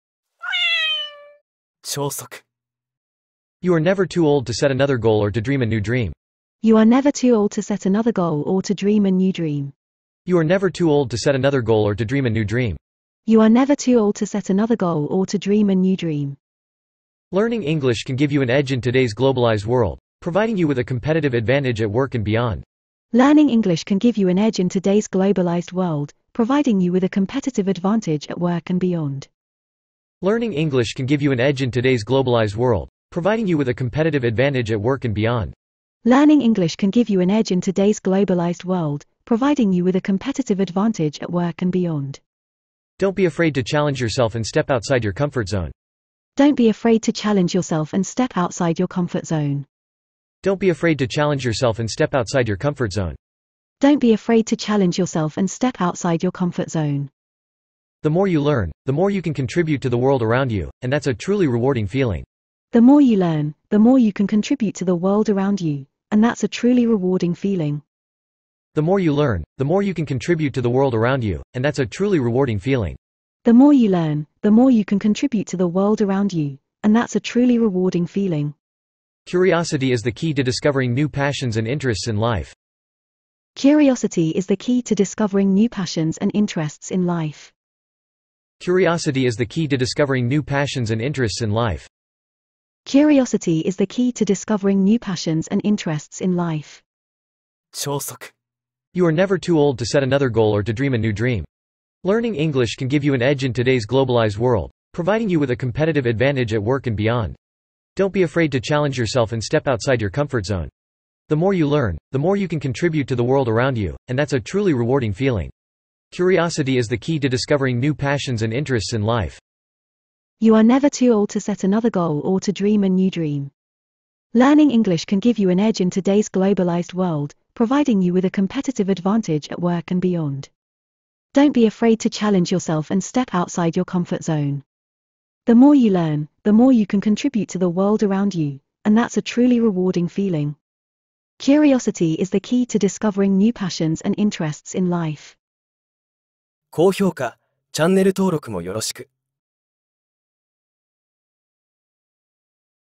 【ポジティブ例文１分だけ】超速英語リスニング_230402
You are never too old to set another goal or to dream a new dream.
新たな目標を設定したり、新たな夢を描いたりするのに、遅すぎることはない。
Learning English can give you an edge in today's globalized world, providing you with a competitive advantage at work and beyond.
英語を学ぶことで、グローバル化した現代社会で優位に立つことができ、仕事やそれ以外の場面でも競争力を発揮することができます。
Don't be afraid to challenge yourself and step outside your comfort zone.
チャレンジすることを恐れず、コンフォートゾーンから一歩踏み出してみてください。
The more you learn, the more you can contribute to the world around you, and that's a truly rewarding feeling.
学べば学ぶほど、周囲に貢献できることが増え、やりがいを感じることができます。
Curiosity is the key to discovering new passions and interests in life.
好奇心は、人生における新しい情熱や興味を発見するための鍵です。